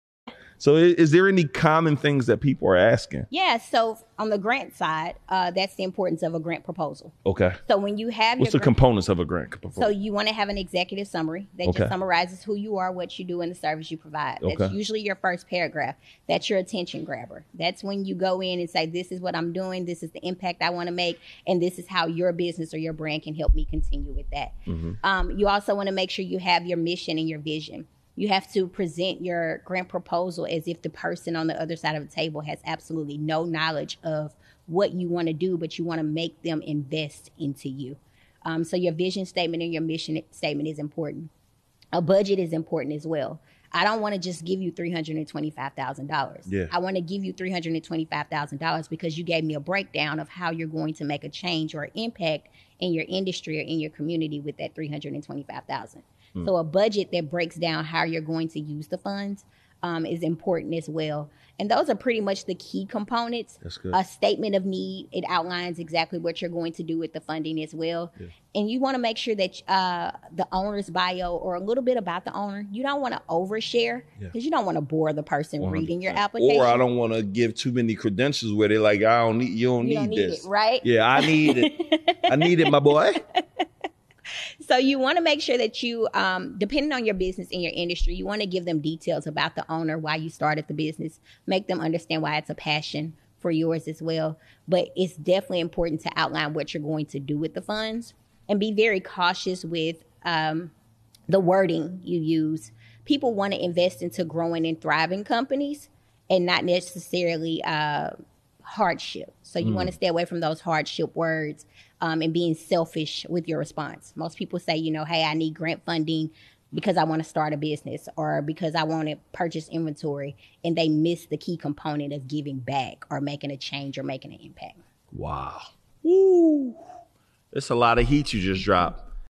Essential Items for Crafting a Successful Grant Proposal
Discover the essential components of a winning grant proposal to boost your chances of securing funding. In this video, we'll break down key elements that make a grant proposal stand out. Whether you're new to grant writing or looking to refine your skills, these tips will guide you towards success.